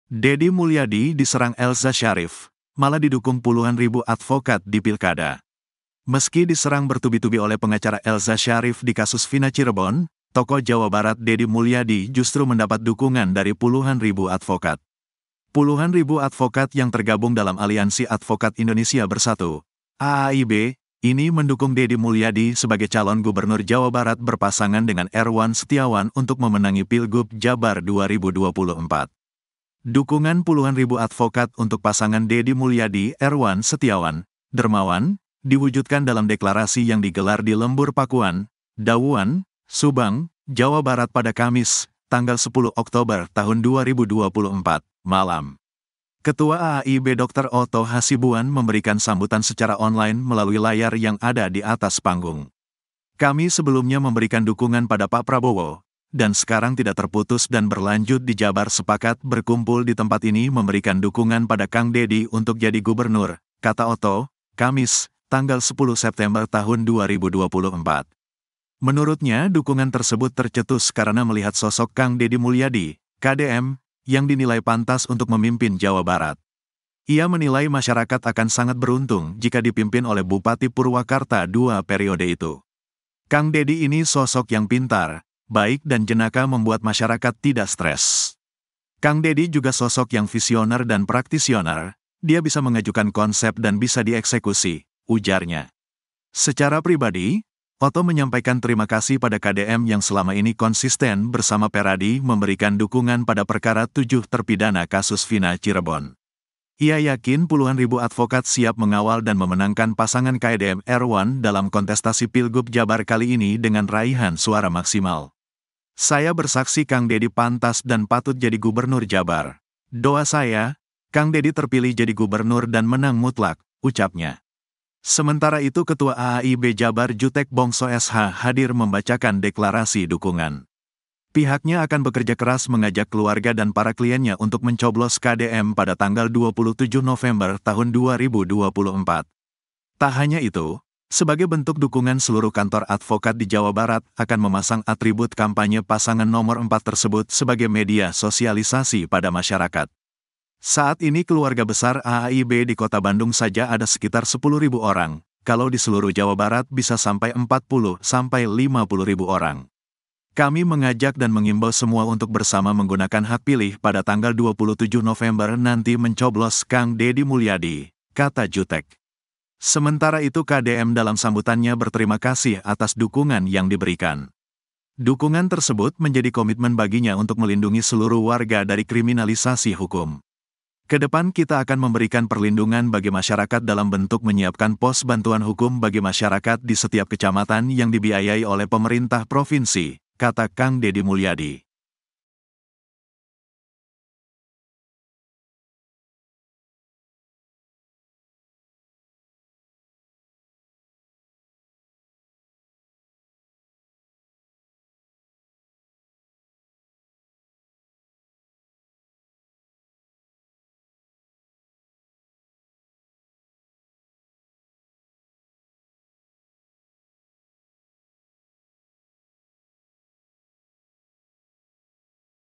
Dedi Mulyadi Diserang Elza Syarief, Malah Didukung Puluhan Ribu Advokat di Pilkada | Tribun Lampung
Dedi Mulyadi Diserang Elza Syarief, Malah Didukung Puluhan Ribu Advokat di Pilkada

Tribun Lampung - Meski diserang bertubi-tubi oleh pengacara Elza Syarief di kasus Vina Cirebon, tokoh Jawa Barat Dedi Mulyadi justru mendapat dukungan dari puluhan ribu advokat.
Puluhan ribu advokat yang tergabung dalam Aliansi Advokat Indonesia Bersatu (AAIB) ini mendukung Dedi Mulyadi sebagai calon Gubernur Jawa Barat berpasangan dengan Erwan Setiawan untuk memenangi Pilgub Jabar 2024.
Dukungan puluhan ribu advokat untuk pasangan Dedi Mulyadi-Erwan Setiawan (Dermawan) diwujudkan dalam deklarasi yang digelar di Lembur Pakuan, Dawuan, Subang, Jawa Barat pada Kamis (10/10/2024) malam.
Ketua AAIB Dr Otto Hasibuan memberikan sambutan secara online melalui layar yang ada di atas panggung.

Editor Video: Tri Prayugo